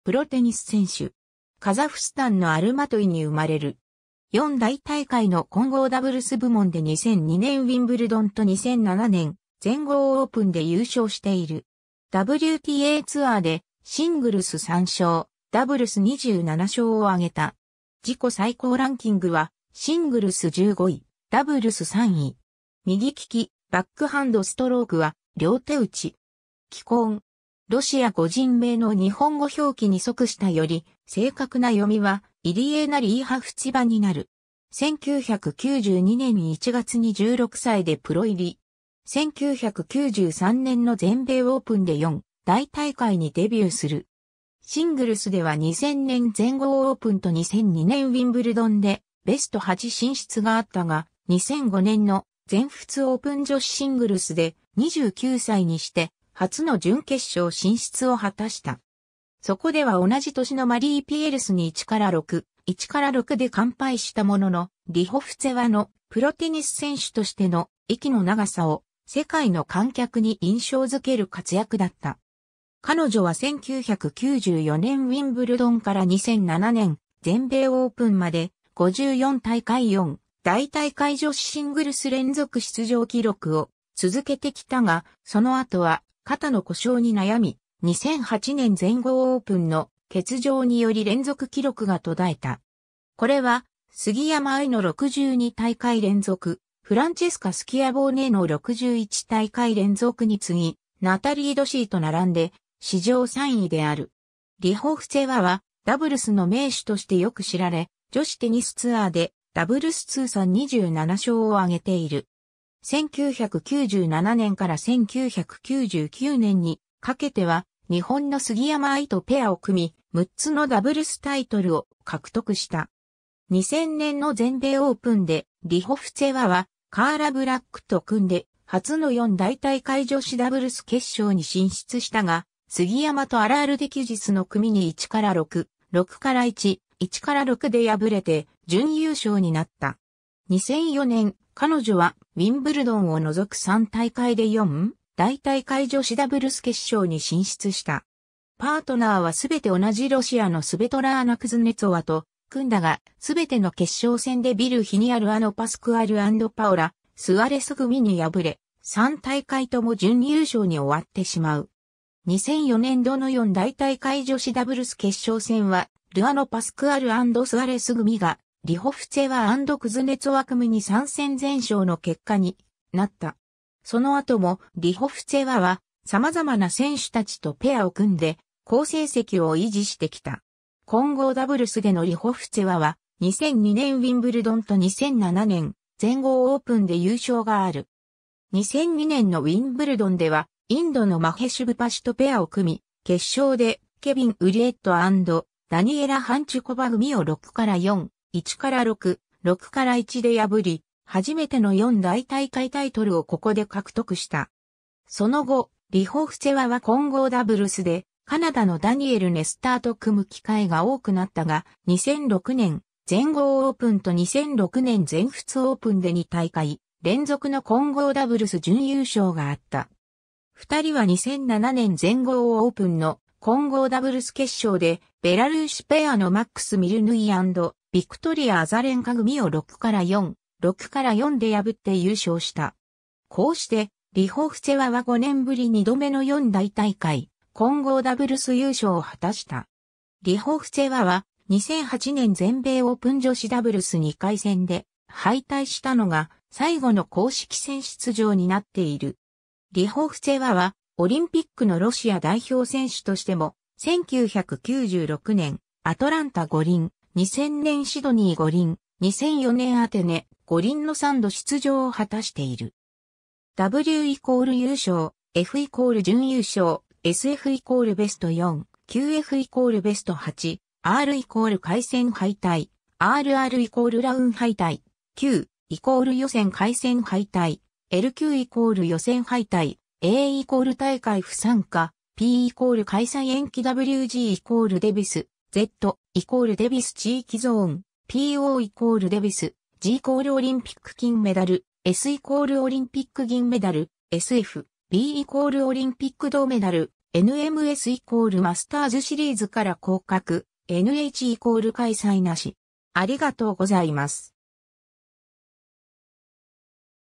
エレーナ・リホフツェワ
エレーナ・リホフツェワ （Elena Likhovtseva, ロシア語: Еле́на Алекса́ндровна Ли́ховцева, 1975年9月8日 - ）は、ロシアの女子プロテニス選手。
カザフスタンのアルマトイに生まれる。
4大大会の混合ダブルス部門で2002年ウィンブルドンと2007年全豪オープンで優勝している。
WTAツアーでシングルス3勝、ダブルス27勝を挙げた。
既婚。
 ロシア語人名の日本語表記に即したより正確な読みは「イリェーナ・リーハフツィヴァ」になる。
 1992年1月に16歳でプロ入り。
1993年の全米オープンで4大大会にデビューする。
シングルスでは2000年全豪オープンと2002年ウィンブルドンでベスト8進出があったが、2005年の全仏オープン女子シングルスで、29歳にして初の準決勝進出を果たした。
そこでは同じ年のマリー・ピエルスに 1-6, 1-6 で完敗したものの、リホフツェワのプロテニス選手としての「息の長さ」を世界の観客に印象づける活躍だった。
彼女は1994年ウィンブルドンから2007年全米オープンまで「54大会」4大大会女子シングルス連続出場記録を続けてきたが、その後は肩の故障に悩み、2008年全豪オープンの欠場により連続記録が途絶えた。
これは杉山愛の62大会連続、フランチェスカ・スキアボーネの61大会連続に次ぎ、ナタリー・ドシーと並んで史上3位である。
 リホフツェワはダブルスの名手としてよく知られ、女子テニスツアーでダブルス通算27勝を挙げている。
1997年から1999年にかけては、日本の杉山愛とペアを組み、6つのダブルス・タイトルを獲得した。
2000年の全米オープンで、リホフツェワはカーラ・ブラック（ジンバブエ）と組んで初の4大大会女子ダブルス決勝に進出したが、杉山とアラール＝デキュジスの組に 1-6, 6-1, 1-6 で敗れて準優勝になった。
2004年、彼女はウィンブルドンを除く3大会で4大大会女子ダブルス決勝に進出した。
パートナーはすべて同じロシアのスベトラーナ・クズネツォワと組んだが、すべての決勝戦でビルヒニア・ルアノ・パスクアル（スペイン）&パオラ・スアレス（アルゼンチン）組に敗れ、3大会とも準優勝に終わってしまう。
2004年度の4大大会女子ダブルス決勝戦は「ルアノ・パスクアル&スアレス組が、リホフツェワ&クズネツォワ組に3戦全勝」の結果になった。
その後も、リホフツェワは様々な選手たちとペアを組んで好成績を維持してきた。
 混合ダブルスでのリホフツェワは、2002年ウィンブルドンと2007年全豪オープンで優勝がある。
2002年のウィンブルドンではインドのマヘシュ・ブパシとペアを組み、決勝でケビン・ウリエット（ジンバブエ）&ダニエラ・ハンチュコバ（スロバキア）組を 6-4, 1-6, 6-1 で破り、初めての4大大会タイトルをここで獲得した。
その後、リホフツェワは混合ダブルスでカナダのダニエル・ネスターと組む機会が多くなったが、2006年全豪オープンと2006年全仏オープンで2大会連続の混合ダブルス準優勝があった。
2人は2007年全豪オープンの混合ダブルス決勝で、ベラルーシペアのマックス・ミルヌイ&ビクトリア・アザレンカ組を 6-4, 6-4 で破って優勝した。
こうして、リホフツェワは5年ぶり2度目の4大大会混合ダブルス優勝を果たした。
 リホフツェワは、2008年全米オープン女子ダブルス2回戦で敗退したのが最後の公式戦出場になっている。
 リホフツェワはオリンピックのロシア代表選手としても、1996年アトランタ五輪、2000年シドニー五輪、2004年アテネ五輪の3度出場を果たしている。
W=優勝, F=準優勝, SF=ベスト4, QF=ベスト8, R=回戦敗退, RR=ラウンドロビン敗退, Q#=予選#回戦敗退, LQ=予選敗退, A=大会不参加, P=開催延期 WG=デビスカップワールドグループ, Z#=デビスカップ地域ゾーン, PO=デビスカッププレーオフ...